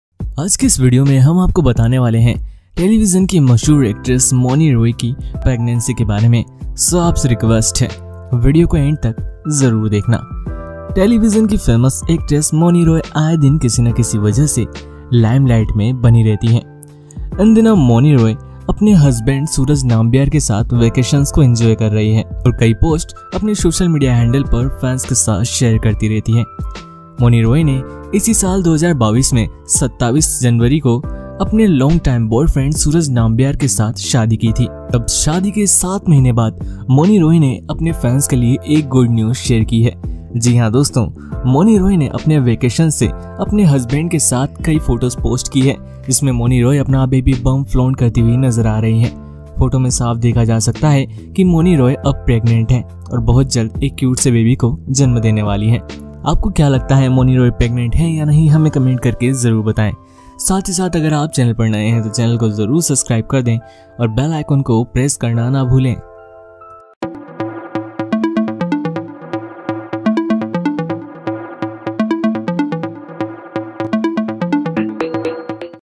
Mouni Roy is Pregnant and Flaunting her Baby Bump with Husband Suraj Nambiar
About Bollywood Buddy -
BollywoodBuddy is an Entertainment Channel that brings you all Trending Updates of the TV and Film Industry and Many More...